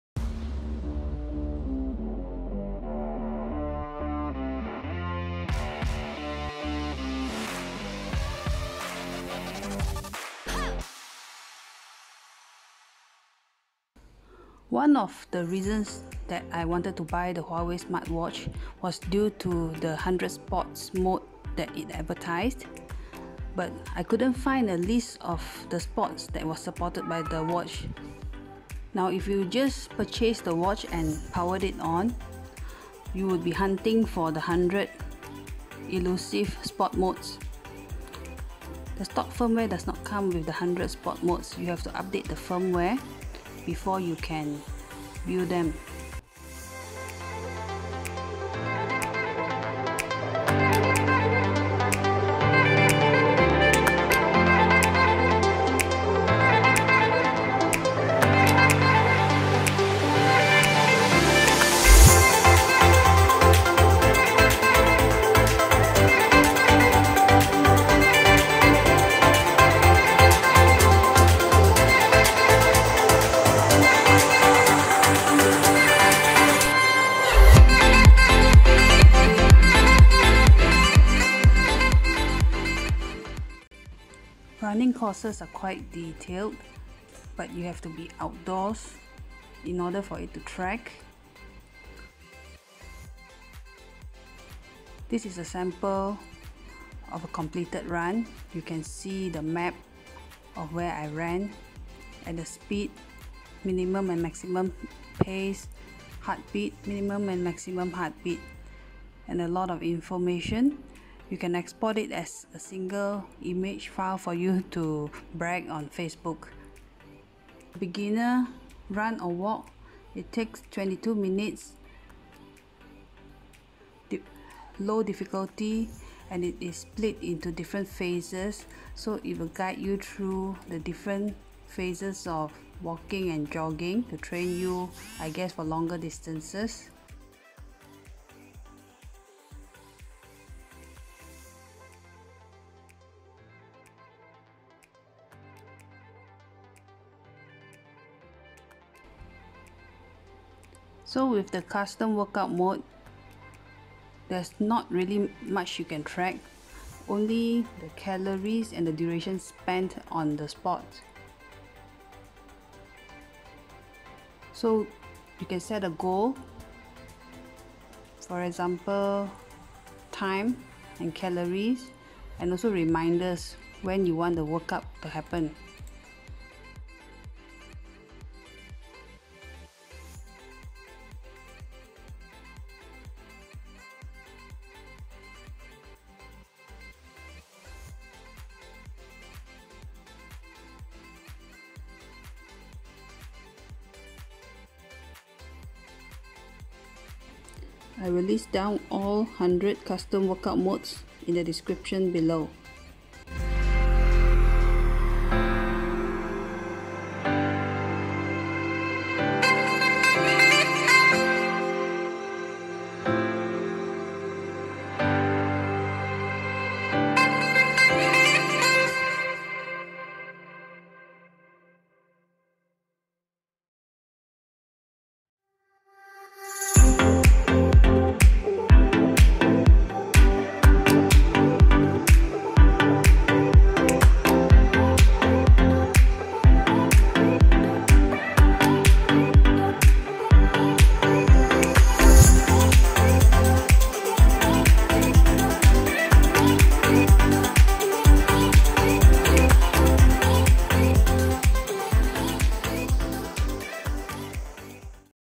Huawei GT2e smart watch 100 sports modes
Complete list of all supported sports mode

Elliptical
Pool Swim
Open water swim

Indoors
Strength
Spinning
Stepper
Air walker
Aerobics
Pilates
Cross Fit
Physical Training
Taekwondo
Free Sparring
Fencing
Belly dance
Jazz dance
Latin dance
Ballet
Core training
Body combat
Kendo
Single Bar
Parallel Bars

Fun
Street Dance
Roller skating
Martial arts
Plaza dancing
Taichi
Dance
Hula Hoop
Frisbee
Darts
Archery
Horse Riding
Hunting
Kite Flying
Tug of war
Swinging
Stair climbing
Obstacle race
Fishing

Sports
Badminton
Table tennis
Tennis
Pool
Bowling
Volleyball
Shuttlecock
Handball
Baseball
Softball
Cricket
Football
Beach soccer
Beach volleyball
Gateball
Hockey
Squash
Sepak takraw
Dodgeball

Water
Sailing
Surfing
Rafting
Dragon boating
Canoeing
Rowing
Water scooter
SUP

Winter
Ice skating
Ice hockey
Curling
Bobsleigh
Sledding
Biathlon

Extreme
Skateboarding
Rock climbing
Bungee jumping
Parkour
BMX
Orienterring
Parachuting
Auto racing